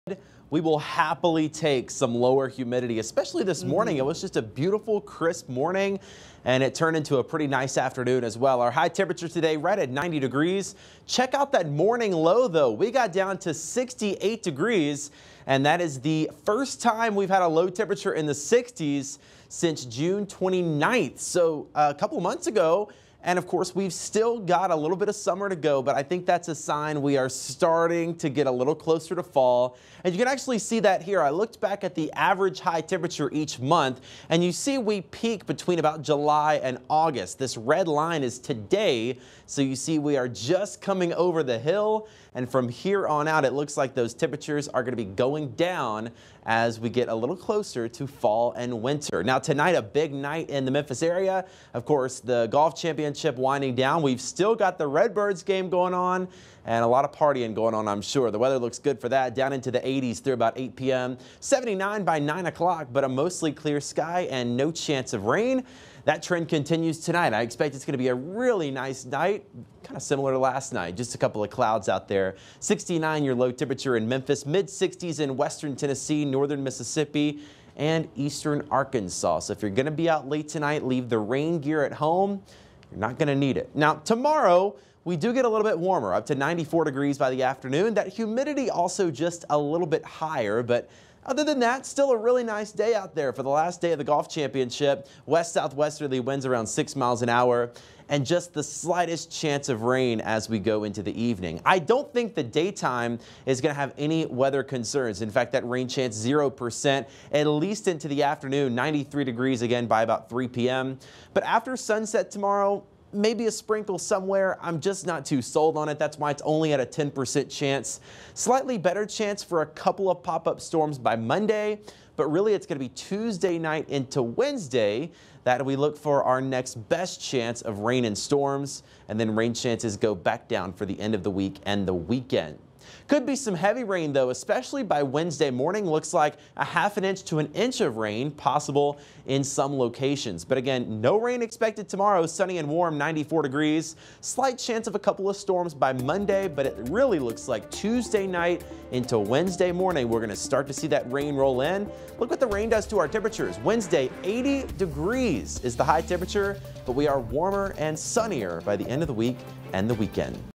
Memphis area forecast: A bit more humid on Sunday
Meteorologist Trevor Birchett says to expect another nice day on Sunday, but it will be warmer. Rain chances return by Tuesday and Wednesday.